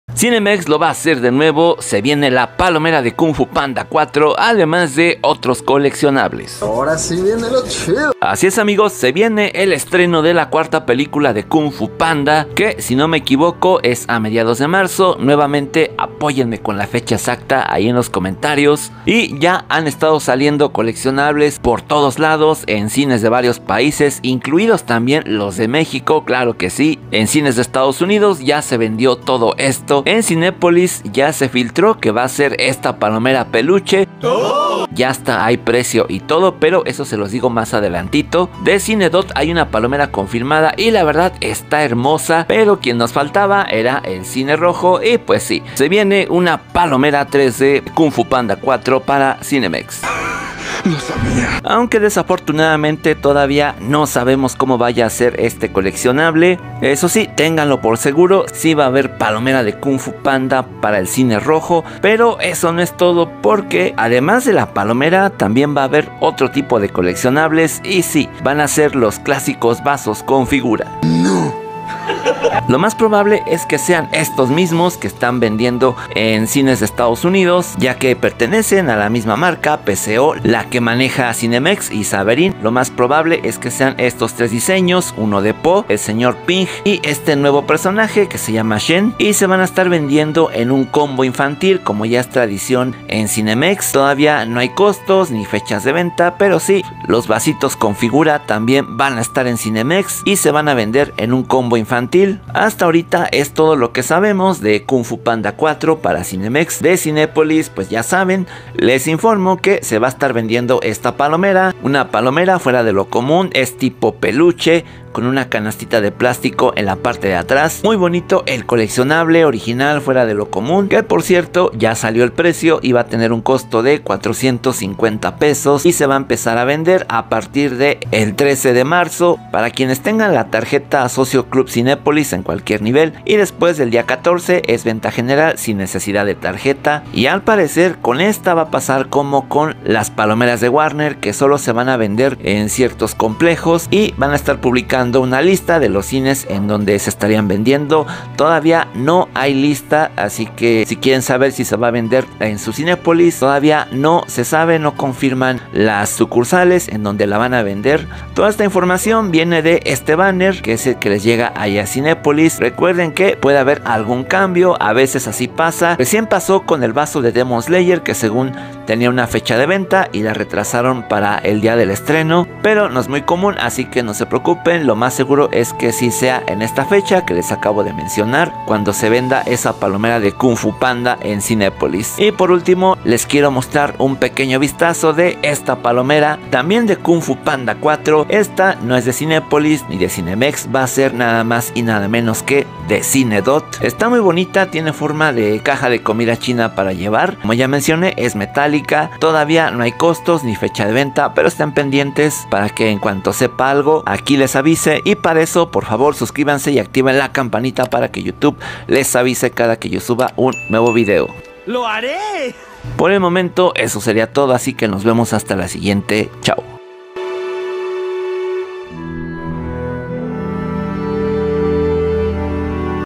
Palomera KUNG FU PANDA en CINEMEX! Y vasos CON FIGURA.
Cinemex sí tendrá una palomera de kung fu panda 4, además de otros coleccionables de cine.